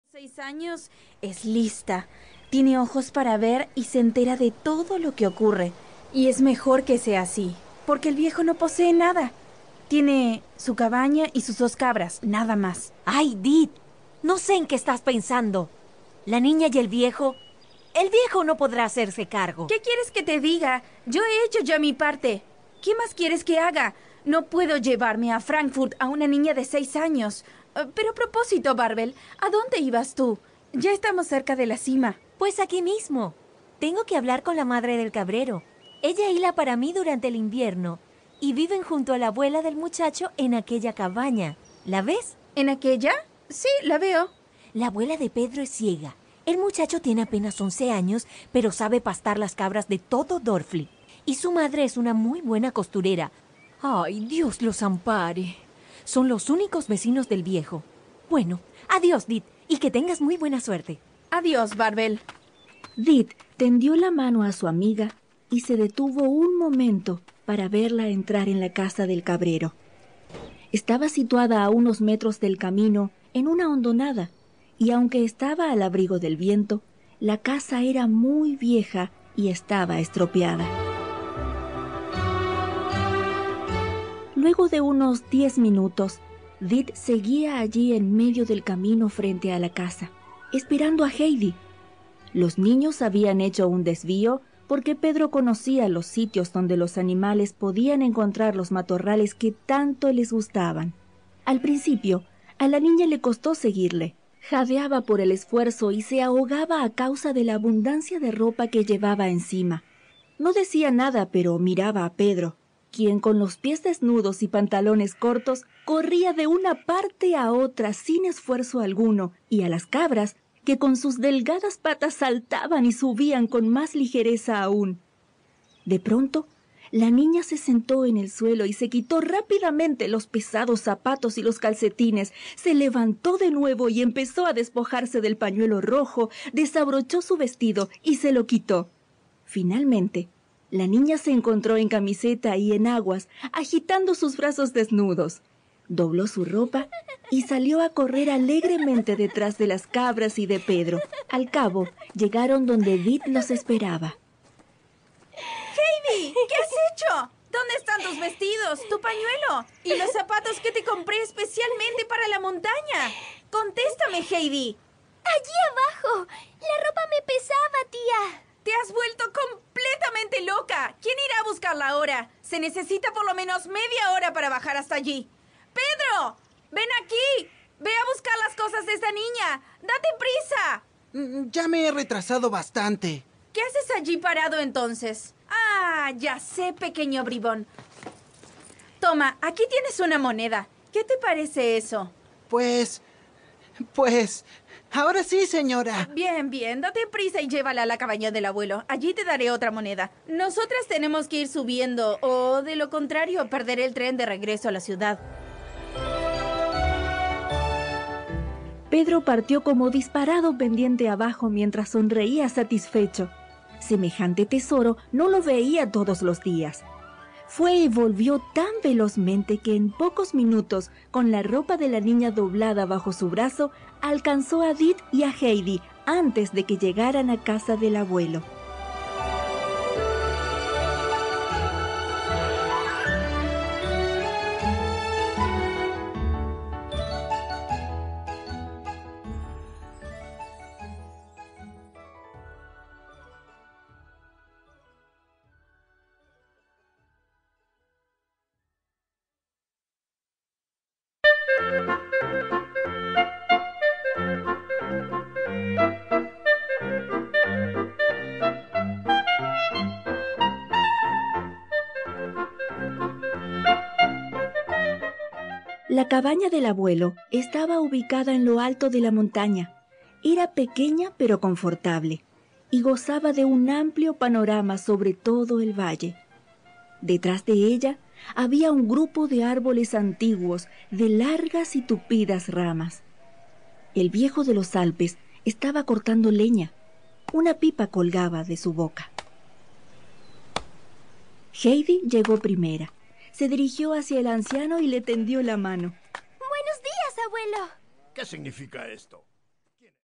HEIDI (Audiolibro) Johanna Spyri (Autor)
Heidi es un libro infantil de 1880 de la escritora suiza Johanna Spyri. Recibe el nombre del personaje protagonista de la historia, Heidi, una pequeña niña que vive en los Alpes suizos cercanos a la frontera con Austria.

Obtenga la versión completa de este audiolibro GRATIS (prueba gratuita de 30 días).